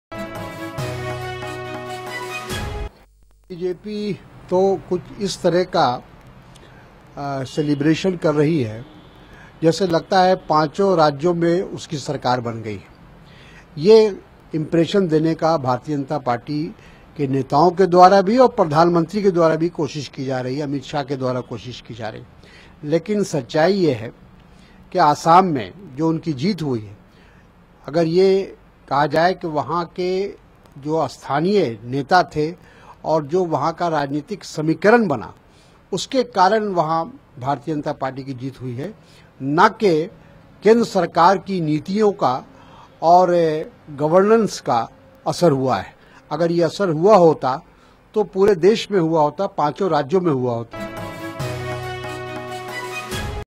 Tariq Anwar on BJP's victory in Assam Vidhansabha Election
NCP leader Tariq Anwar statement on BJP's victory in Assam Vidhansabha Election...